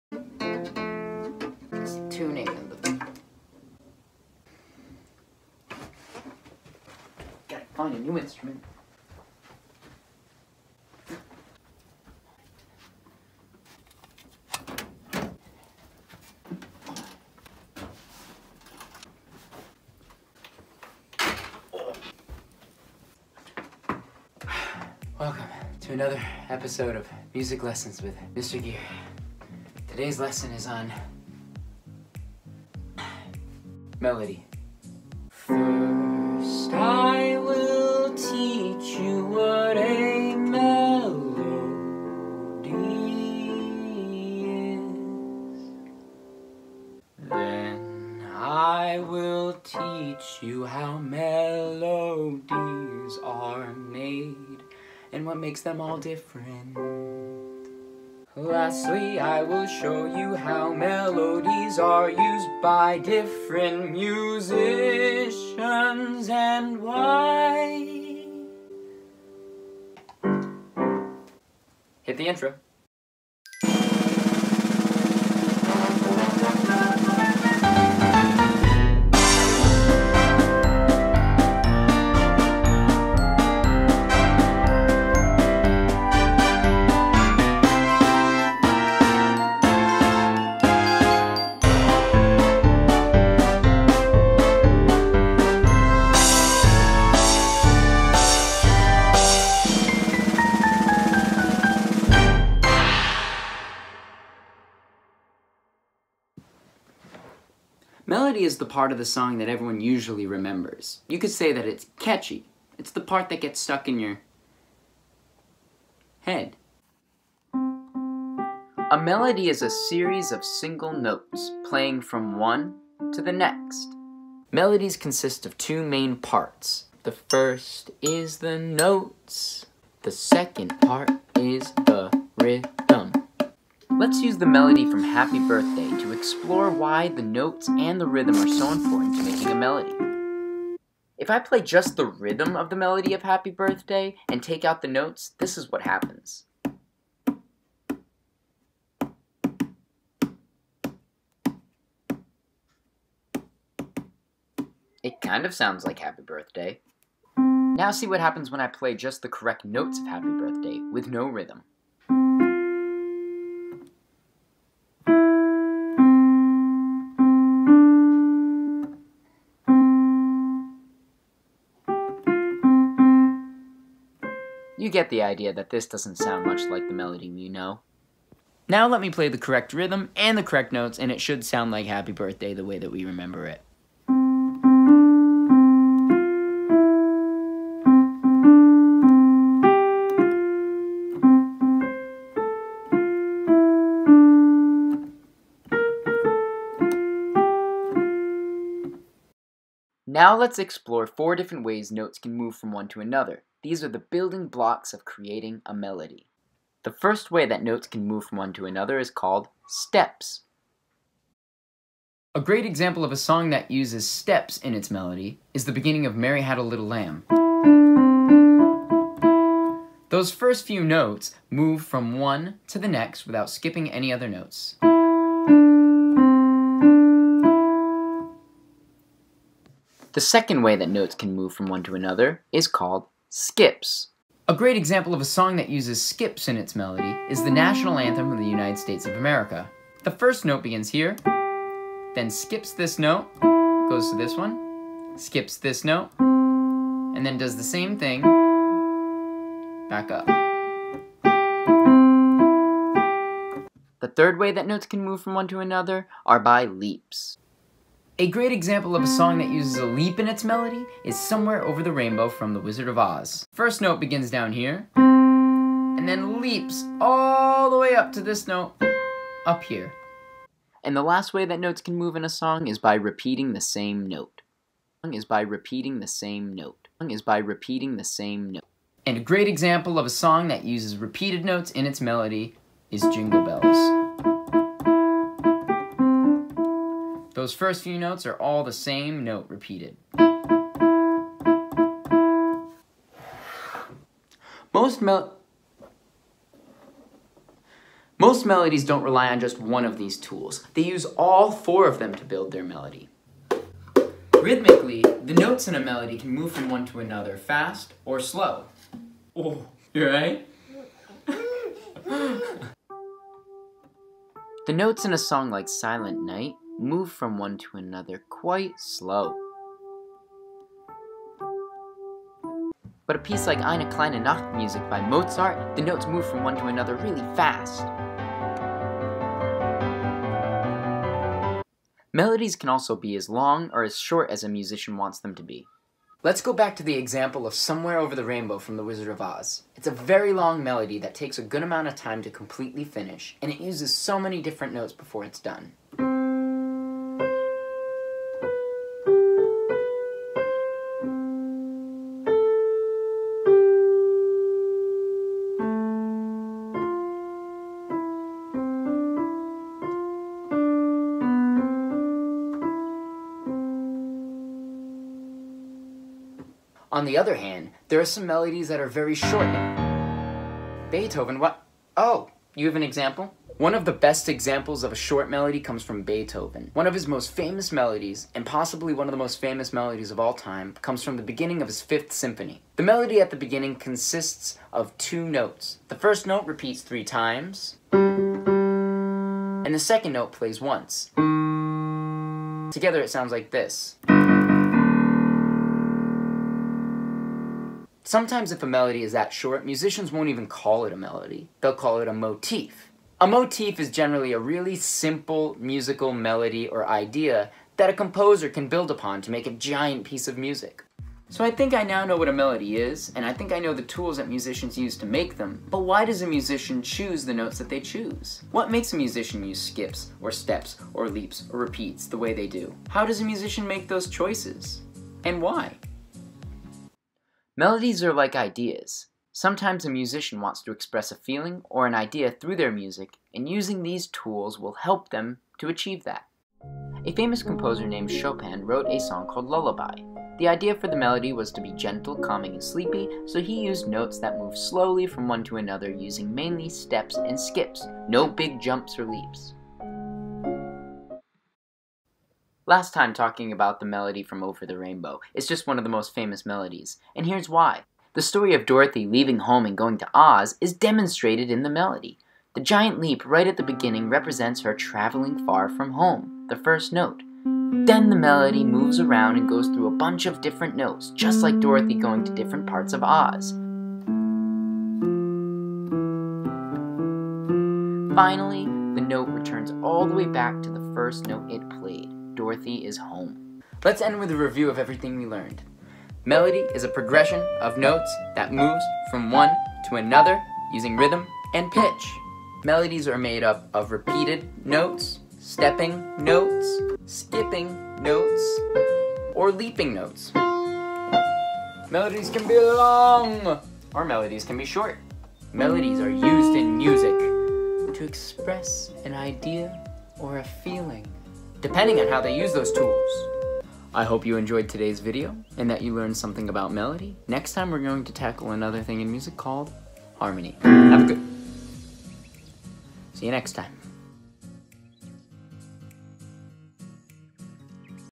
Music Lesson with Mr. Geer- LESSON 3: MELODY
In this video Mr. Geer teaches a lesson about melody !